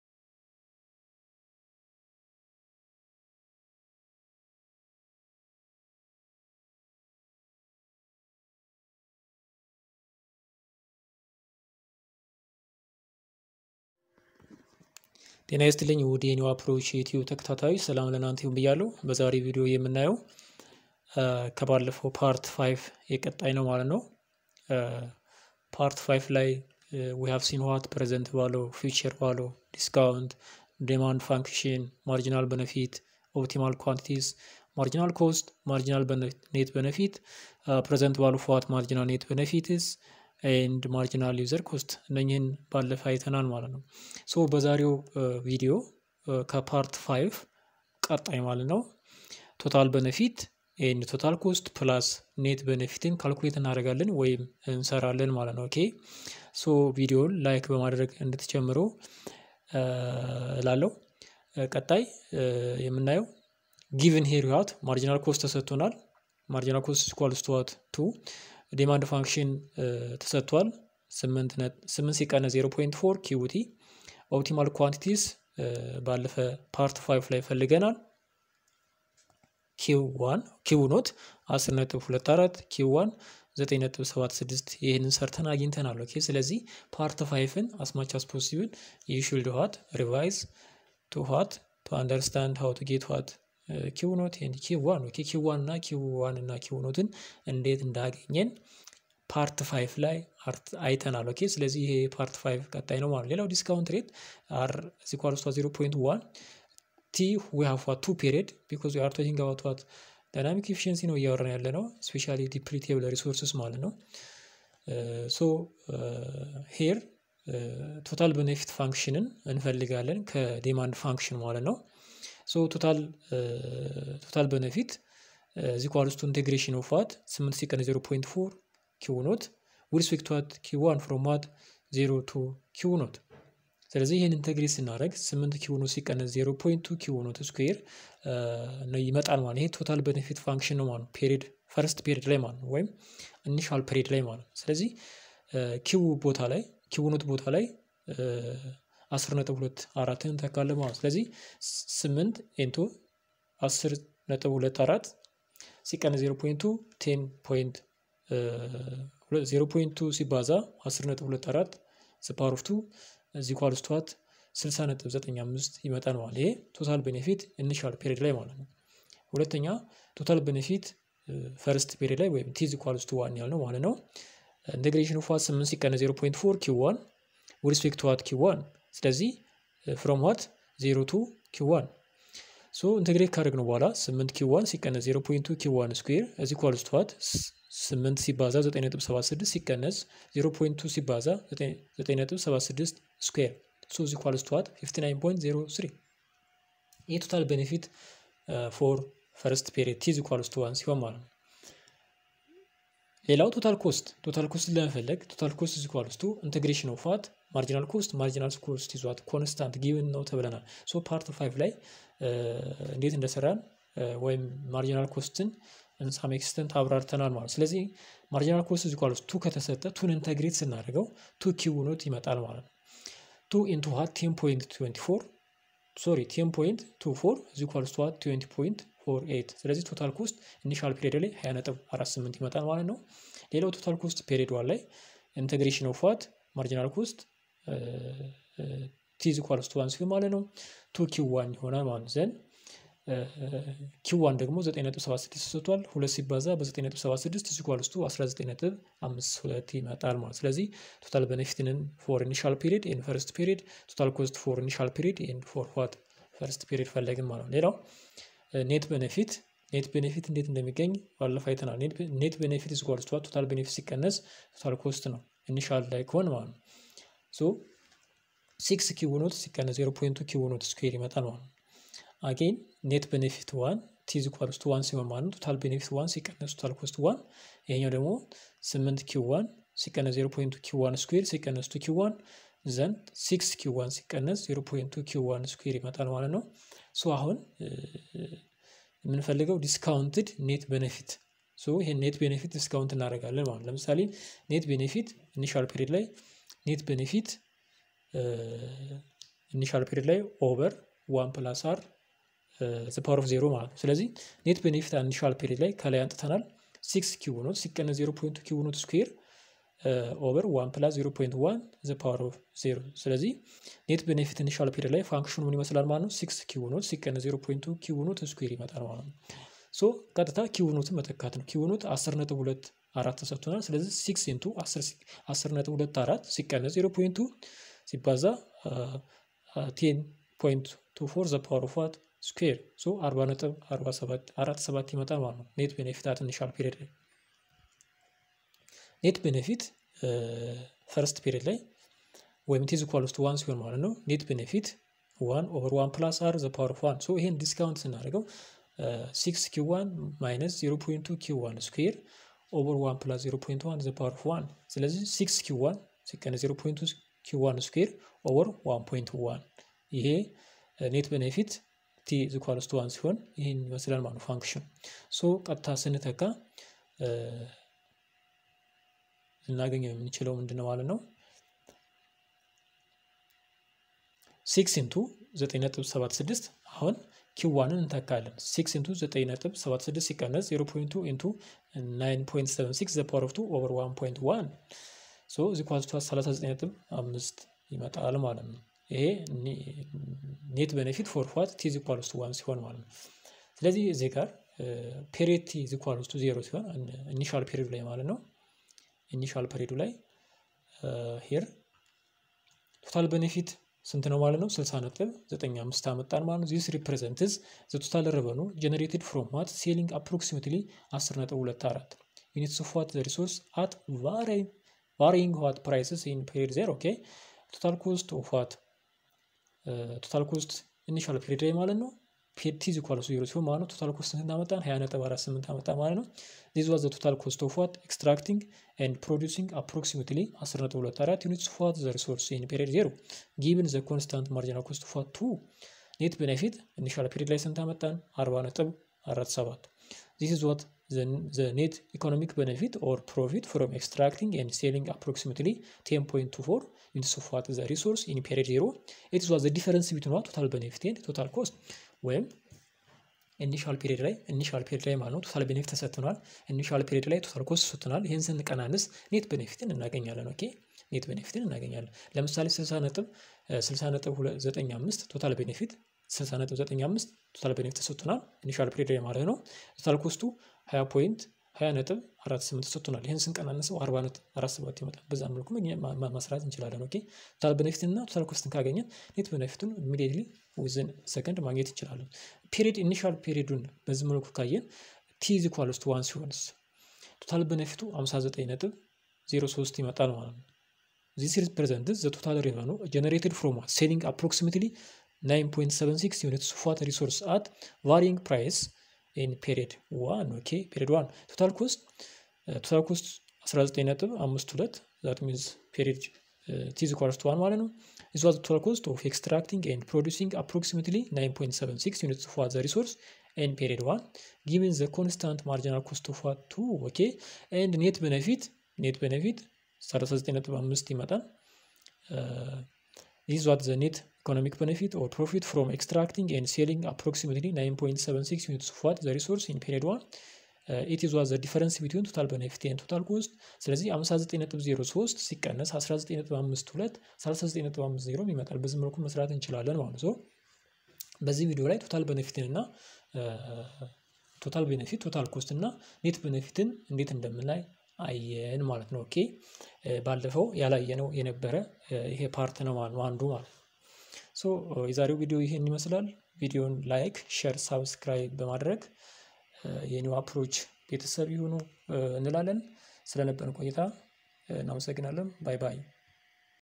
Part-5.1-Environmental economics| Exit Exam Practice part5.1 | tutorial
exit exam | economics | econometrics lecture note | amharic
This is Exit Exam Practice for economics Students
This video includes images, music, clip art, and short videos sourced from Google.